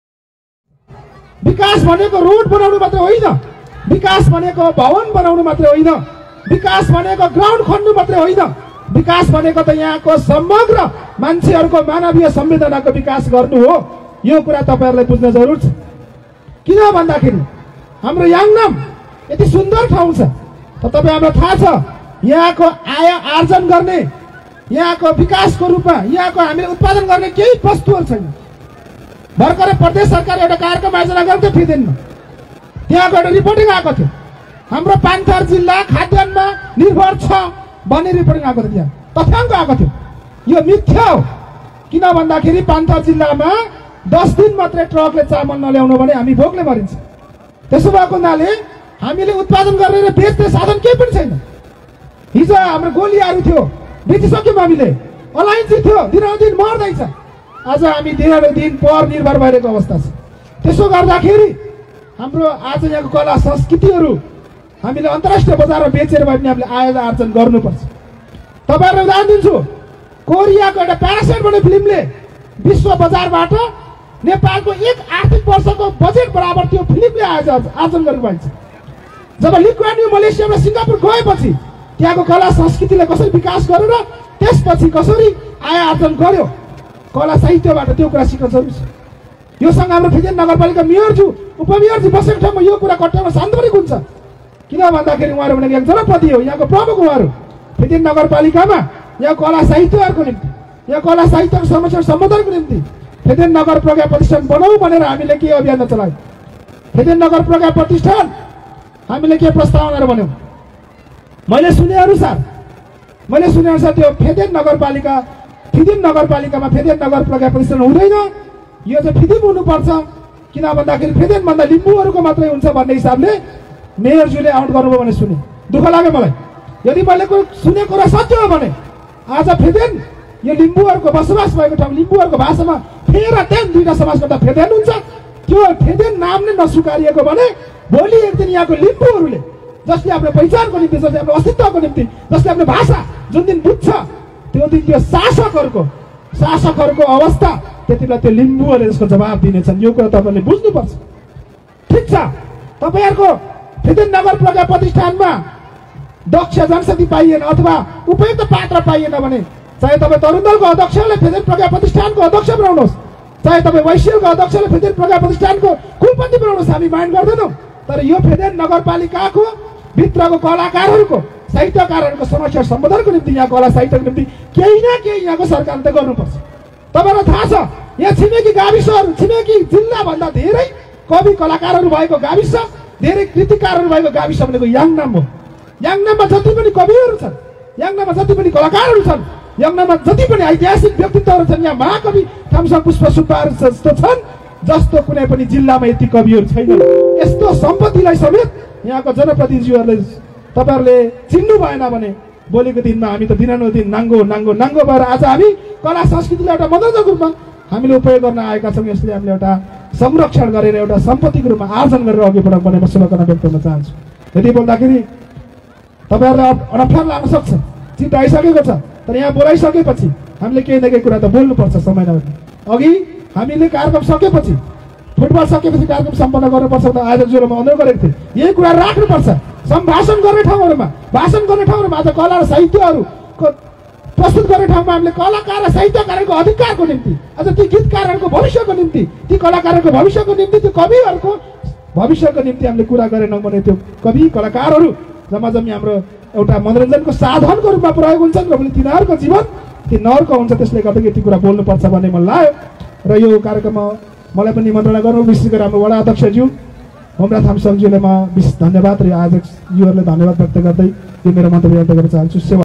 'फेदेन' नाम नस्विकार्ने मेयर,उपमेयरलाइ चेतावनी, एक दिन लिम्बुहरुले बुझेको दिन जवाफ दिन्छन - सागर
याङ्नाम फुङ्नाम मेला २०८१ बैशाख १ गते खलंगाटार याङ्नाम